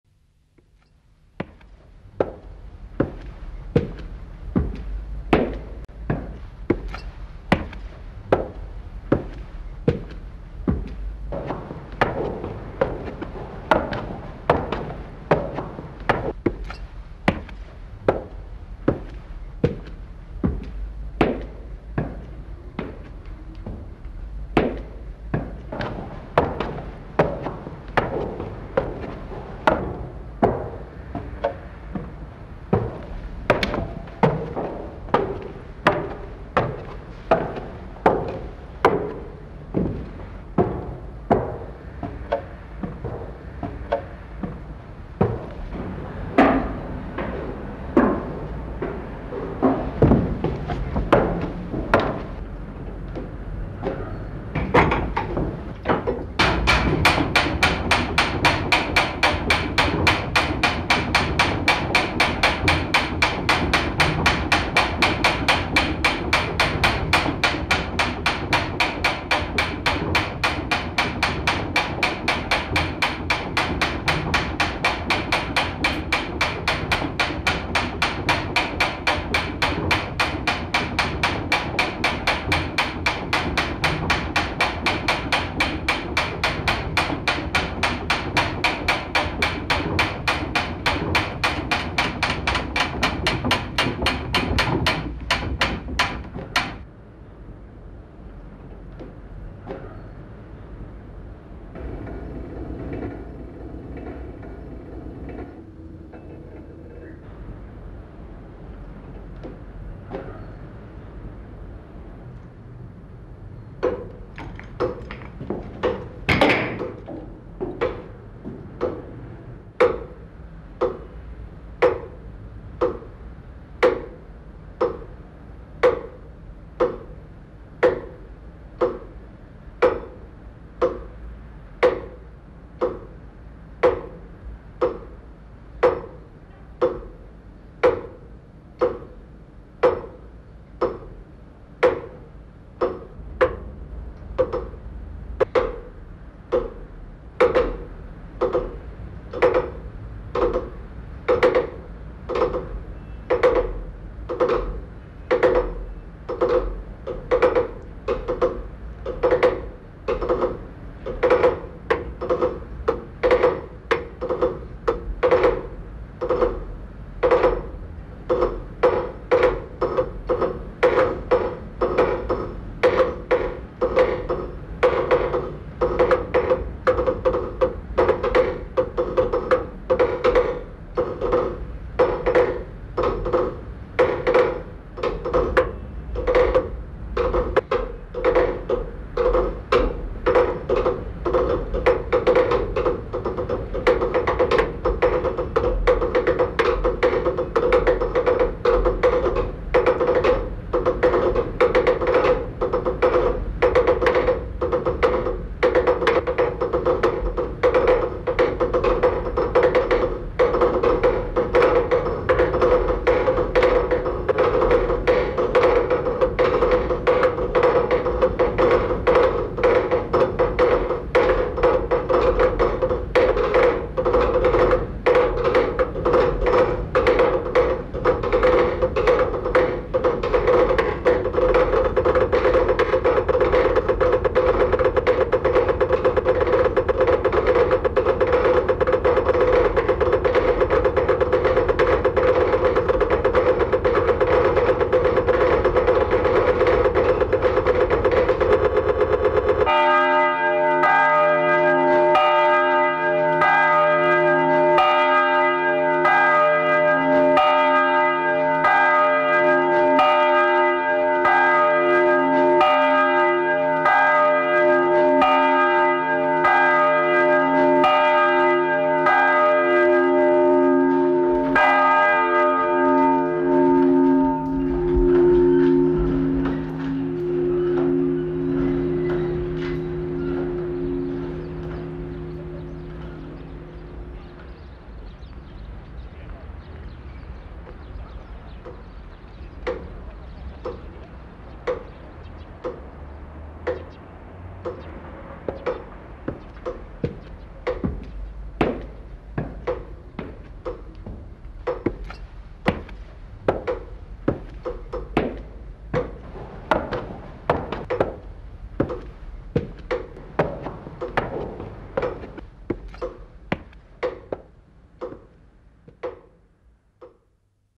A szerkezet....mpg
Esszéfilm, a kolozsvári Szent Mihály Templom toronyórájának szerkezete.